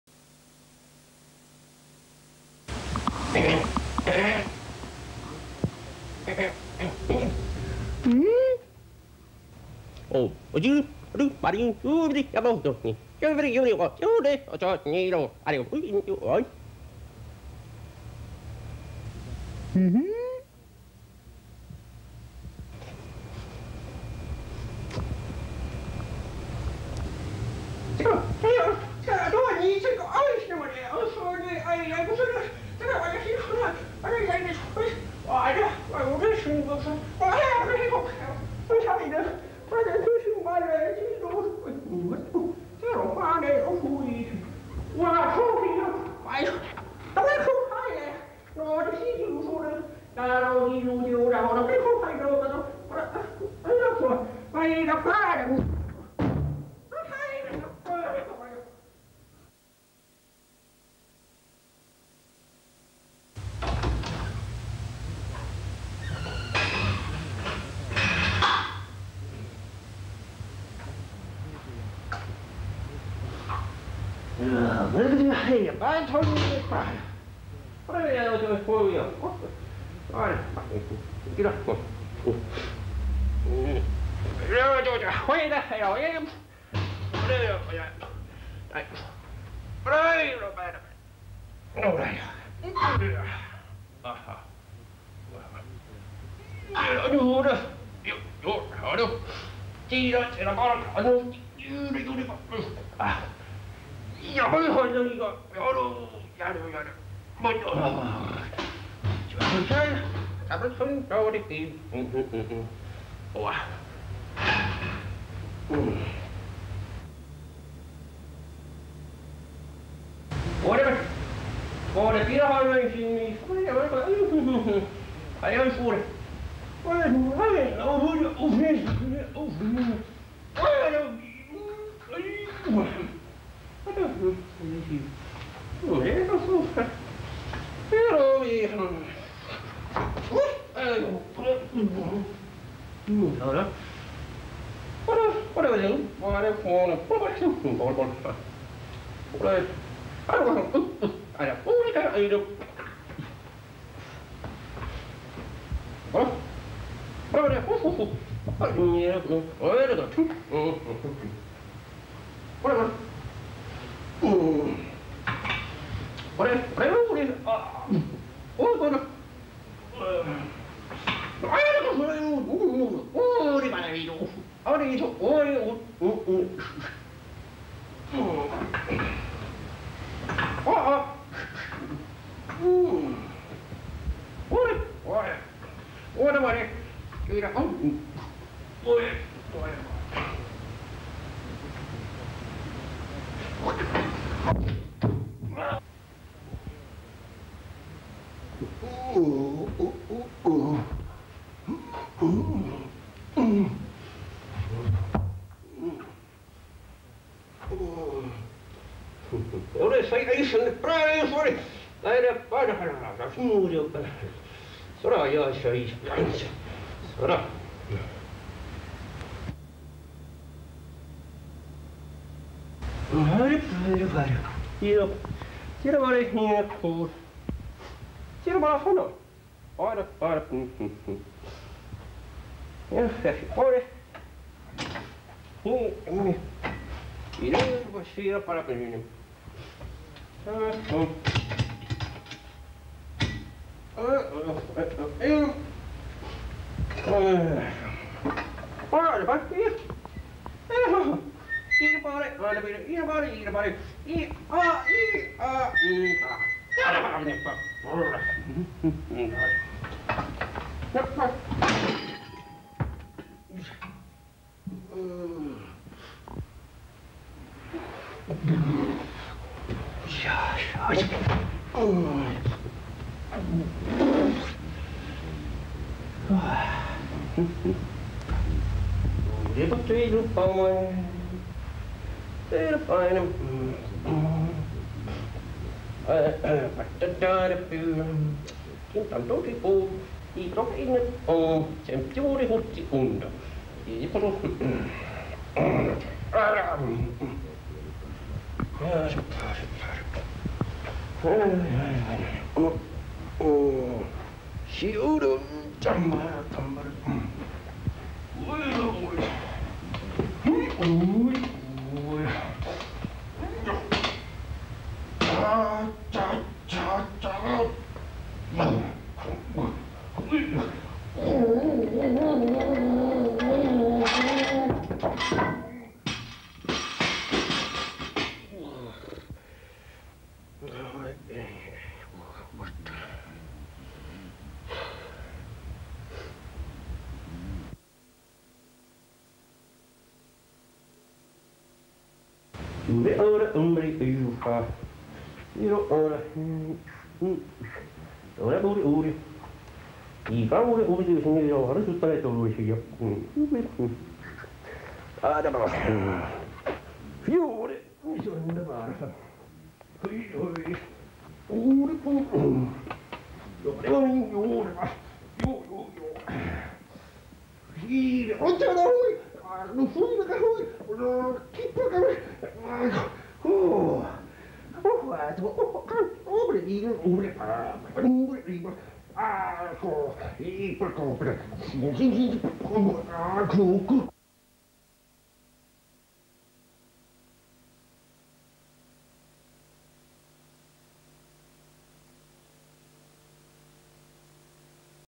Aimo Käki salilla 1988. Westmedia Oy.
Timo Julkunen tuo Pirunpellon ilkeä itämies oli edellisessä elämässään nimeltään Aimo Nils Käki. Aimo seikkaili 80-luvulla Rauma TV:ssä aikansa ja tänään Aimo kokeilee punttisalitoimintaa. kuvaus & edit. Westmedia Oy. Laatua jo vuodesta 1988.